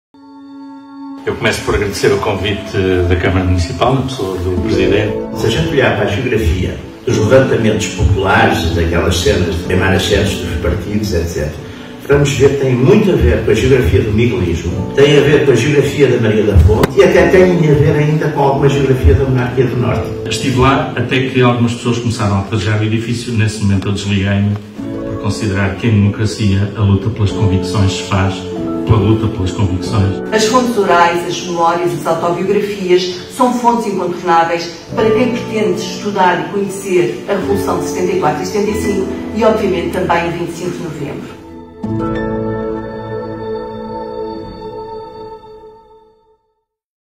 Colóquio "25 de novembro de 75"
Foi com uma sala praticamente cheia que decorreu o colóquio "O 25 de novembro de 75", organizado pela Câmara Municipal de Aveiro.
Um encontro, presidido pelo Presidente da CMA, Ribau Esteves, e que contou com a presença de Maria Inácia Rezola, Jaime Nogueira Pinto e António Marujo, palestrantes convidados.